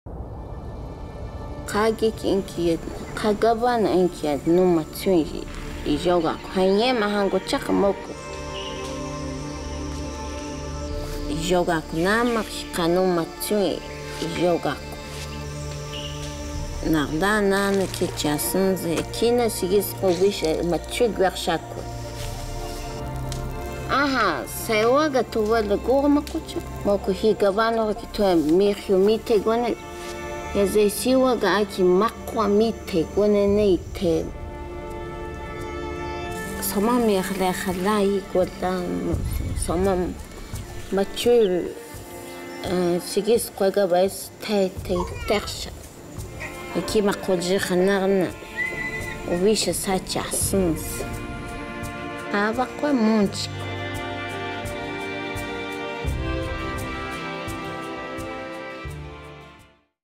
¡Arriba mujeres que regalan vida! | Falabella Colombia
¡Arriba Isabel! Arriba Mujeres que cosechan la tierra y cultivan la vida.
Arriba todas las mamás que viven para amar y respetar. Esta es la visión de la historia de Isabel para Falabella.

☏ Venta telefónica Bogotá 587 8008, Nacional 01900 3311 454.